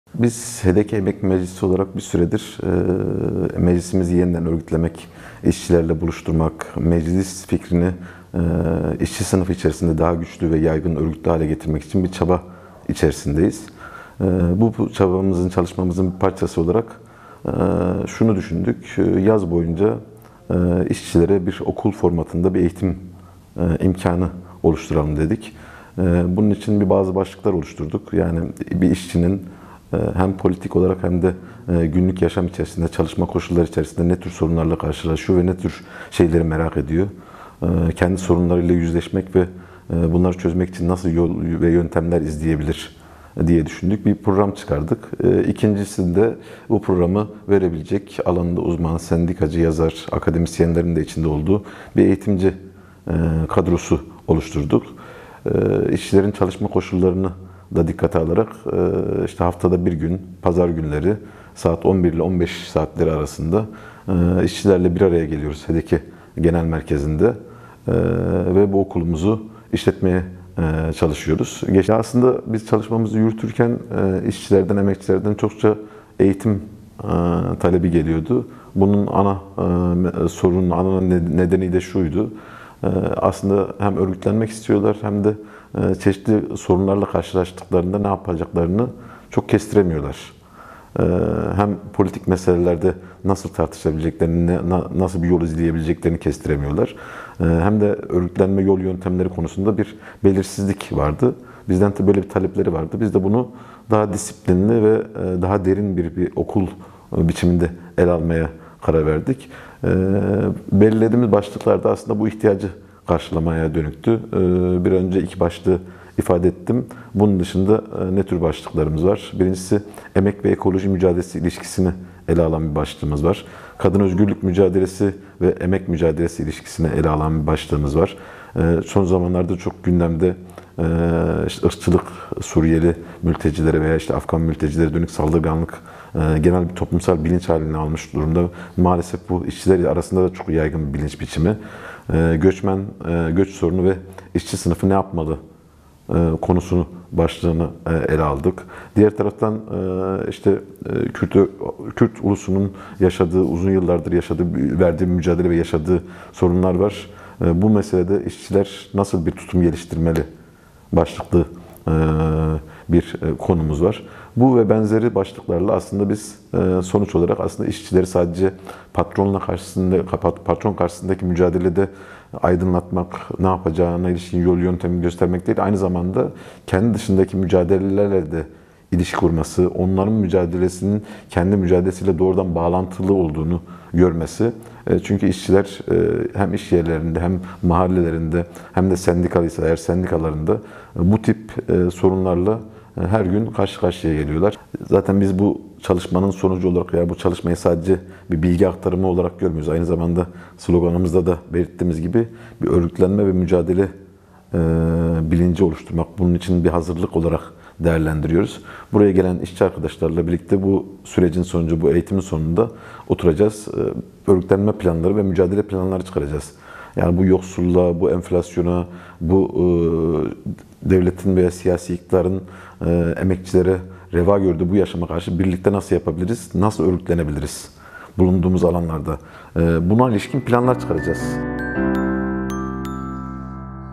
HDK İŞÇİLER İÇİN 'YAZ OKULU PROGRAMI' BAŞLATTI
HDK Emek Meclisi, işçilerin hakları konusunda bilinçlendirmeyi ve işçilerin toplumsal mücadeleye katılımını hedefleyen "Yaz Okulu Programı" başlattı.